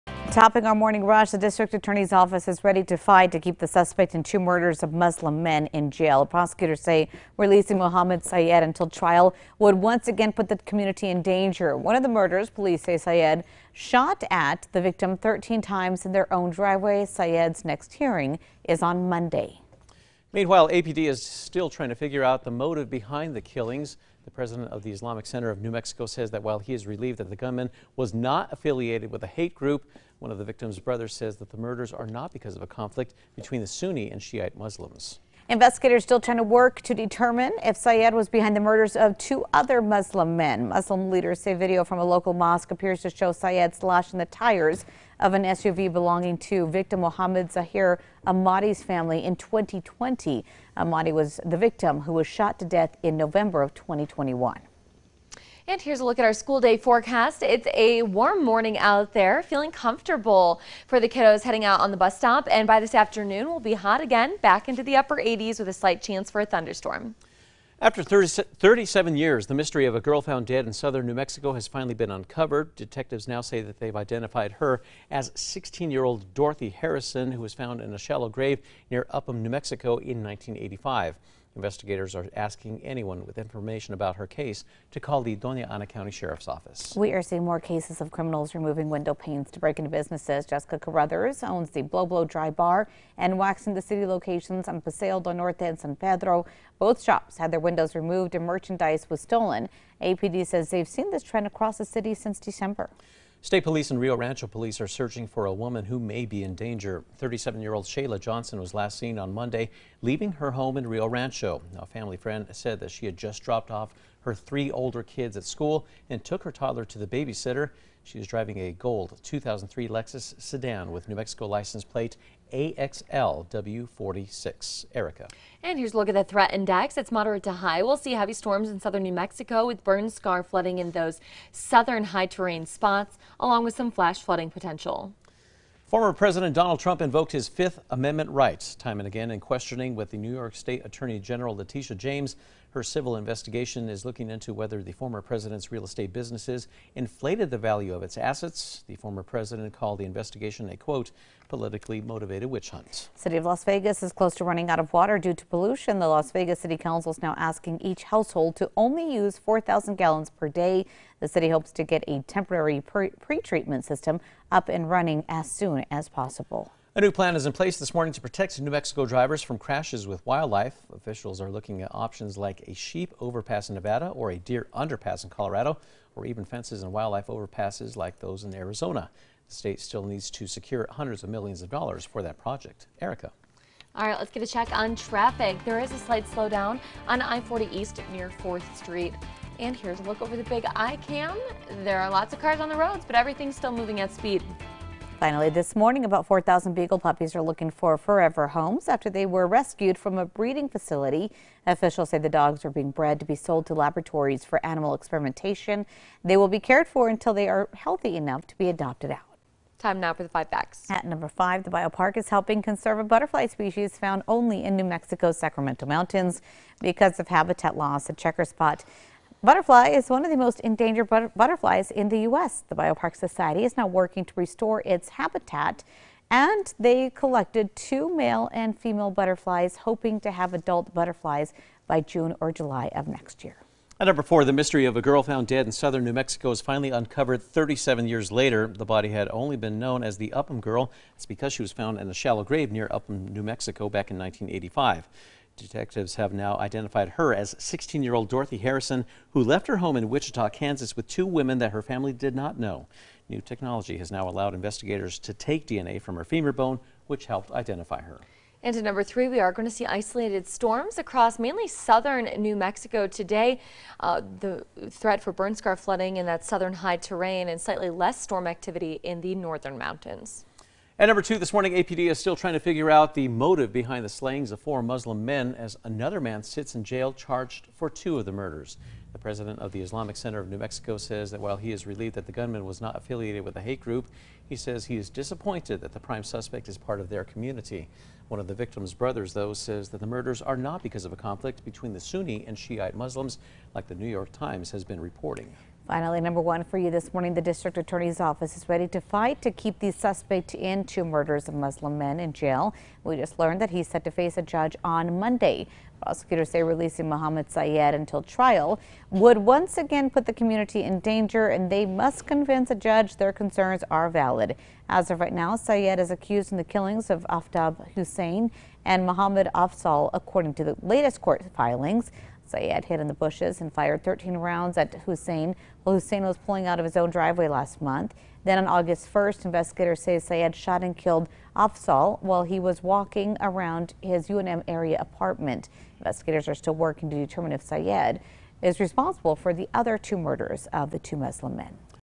KRQE Newsfeed
KRQE Newsfeed: Murder suspect pretrial hearing, Determining a motive, Isolated storms, Girl identified, Conserving butterflies